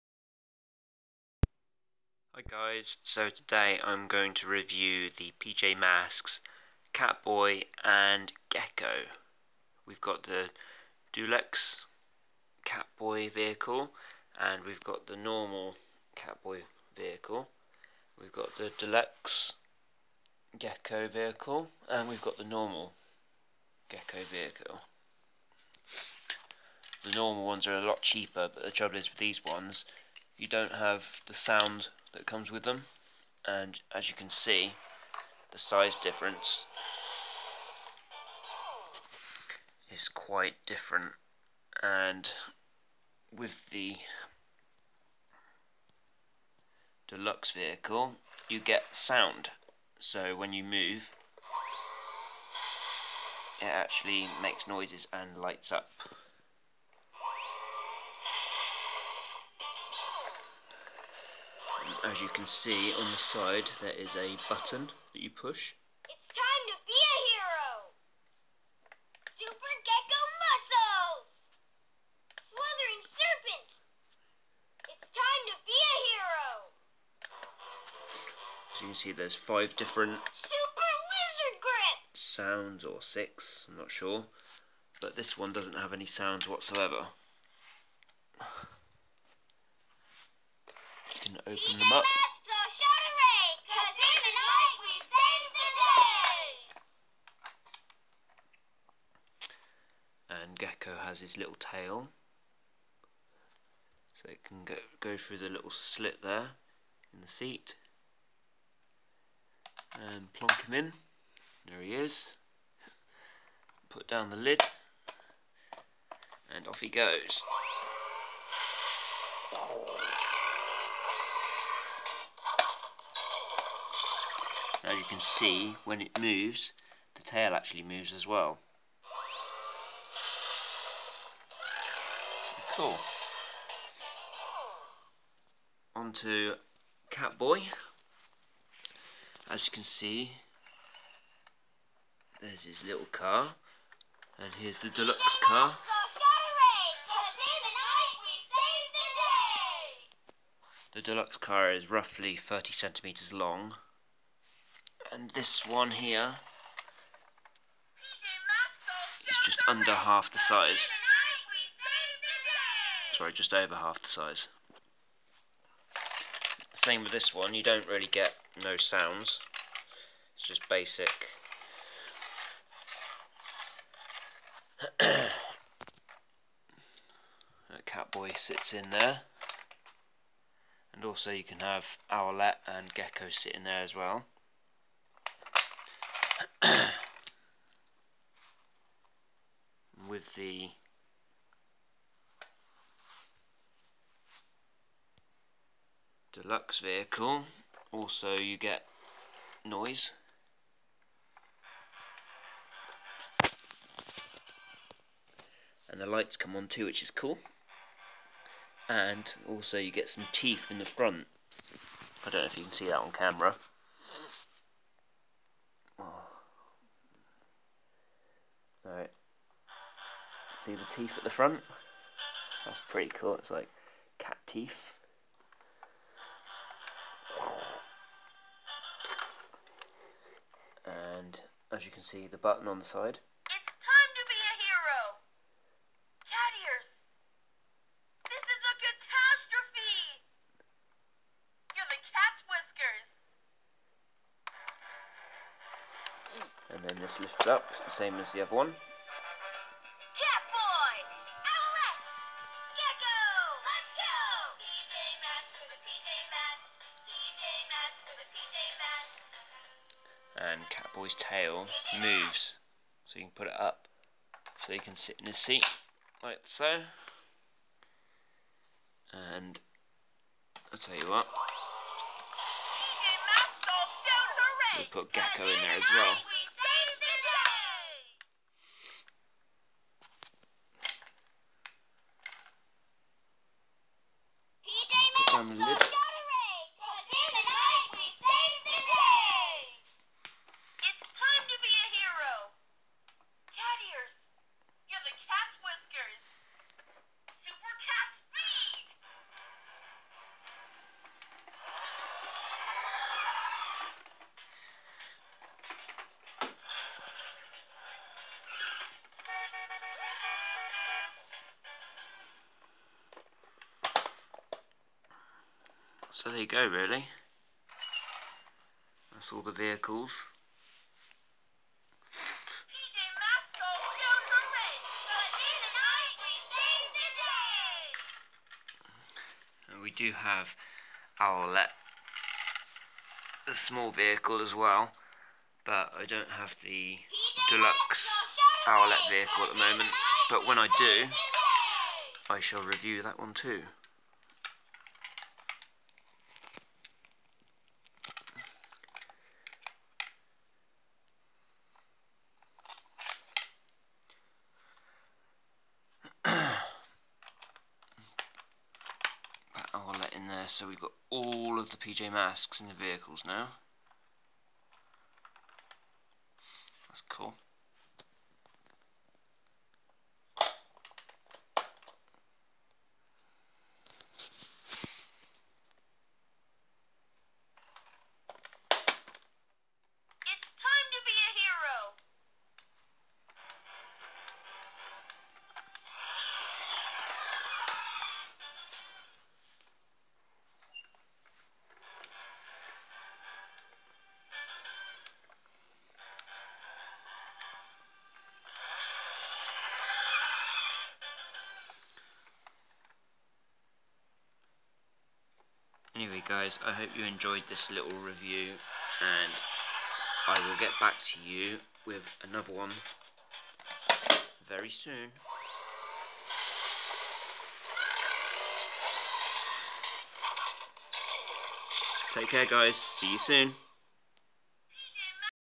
PJ Masks deluxe vehicles CatBoy & Gekko
A look at the dulex & basic PJ Mask vehicle's Cat Boy & Geko